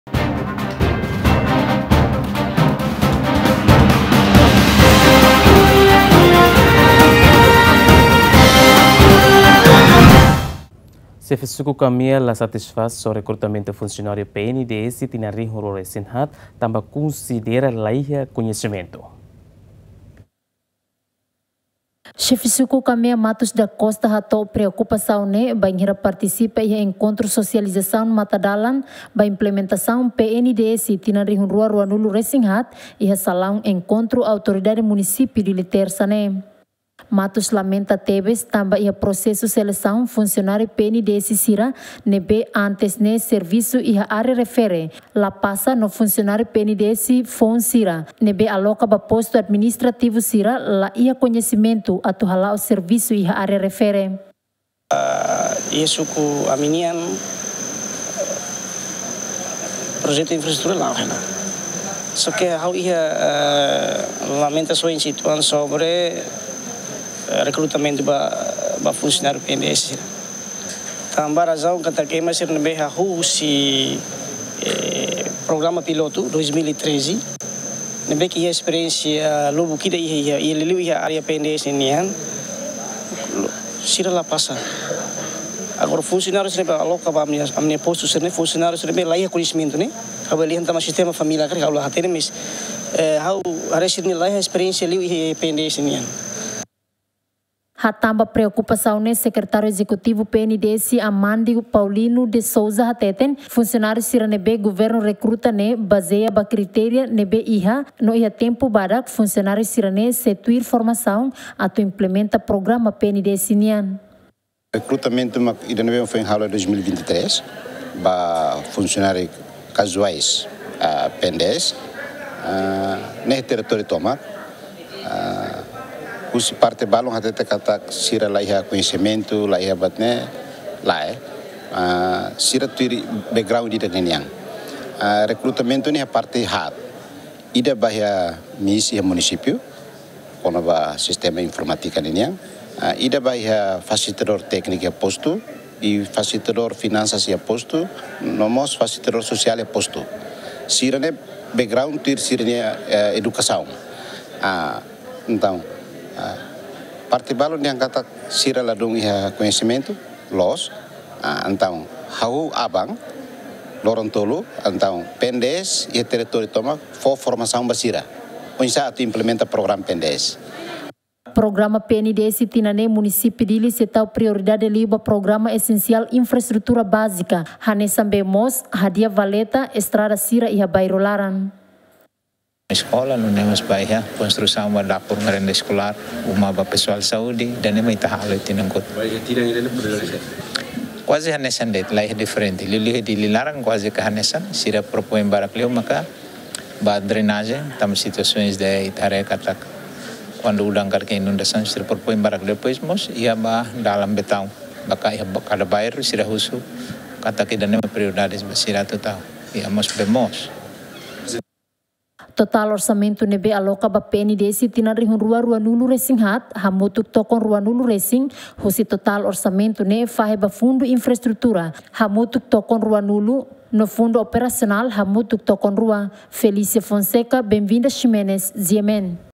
Xefe Suku Kamea la satisfaz ho rekrutamentu funsionáriu PNDS tinan 2024
Xefe Suku Kamea la satisfaz ho rekrutamentu funsionáriu PNDS tinan 2024 tanba konsidera laiha kuñesimentu